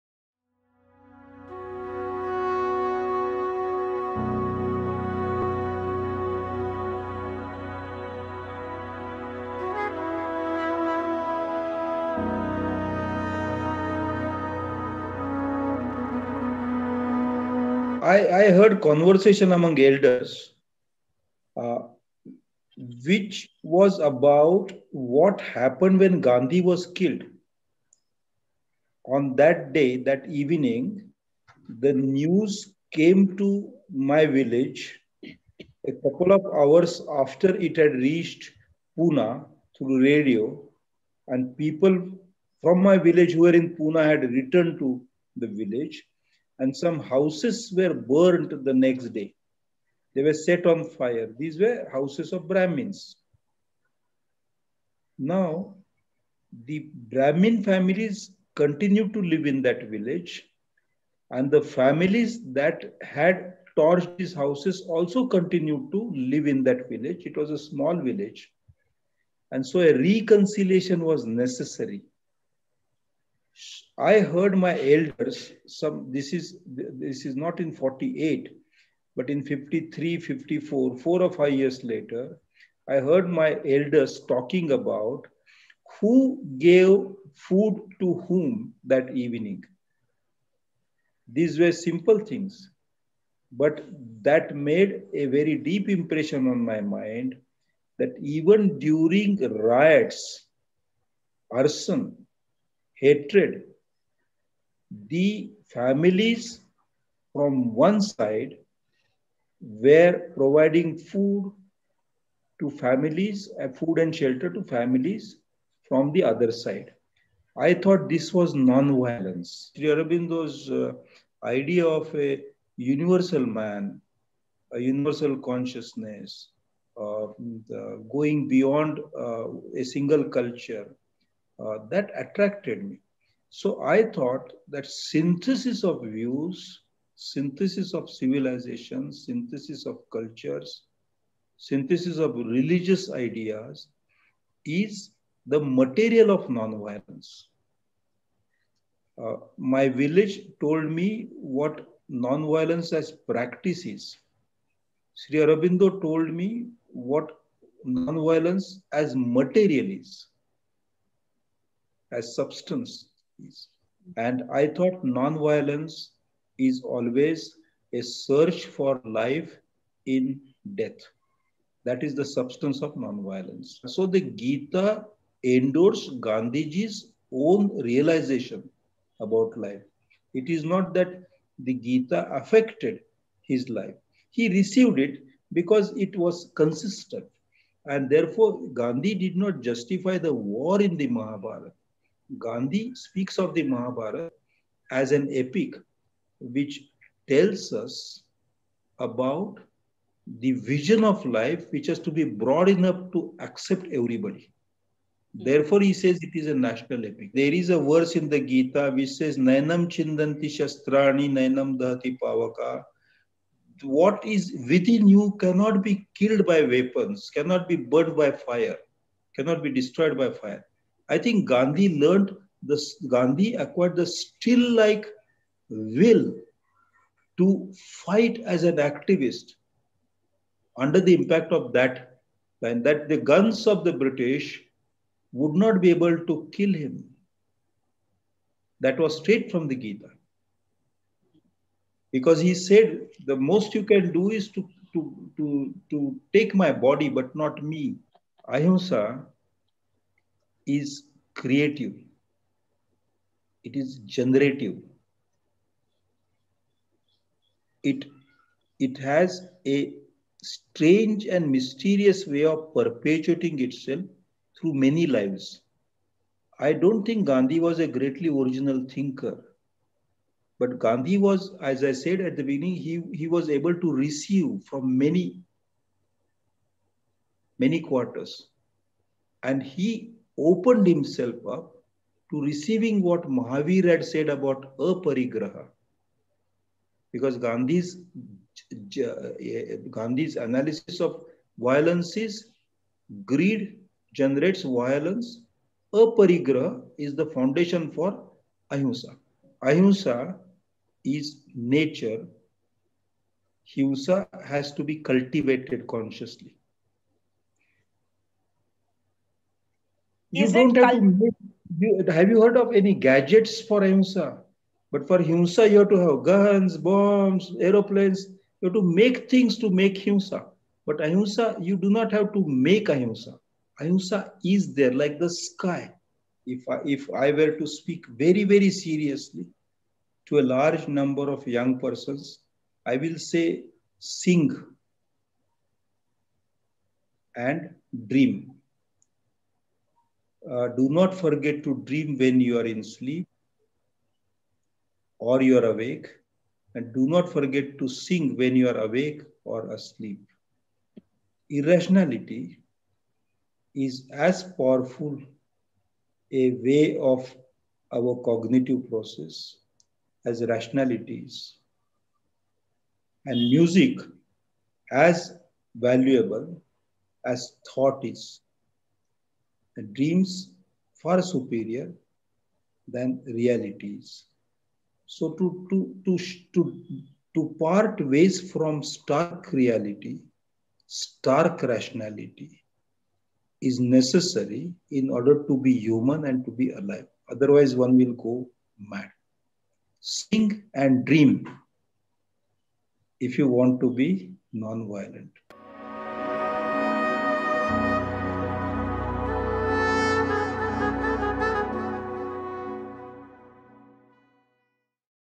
Snippets: Ahimsa Conversation # 41 Ganesh Devy
Dakshinayan, a group of writers, is based on the resolve that nonviolence is fearlessness. Ganesh Devy, scholar, linguist and cultural activist, talks about how the murders of leading Indian intellectuals led to the formation of Dakshinayan. He draws on his childhood memories to describe how nonviolence took shape in his consciousness. Diverse influences, from Sri Aurobindo, the Srimad Bhagvad Gita and Mahatma Gandhi enabled him to understand why nonviolence is creative and natural, whereas violence has to be cultivated. Ganesh appeals for us not to be limited by the stark reality because, he says, those who do not sing and do not dream end up in the mental ward of violence.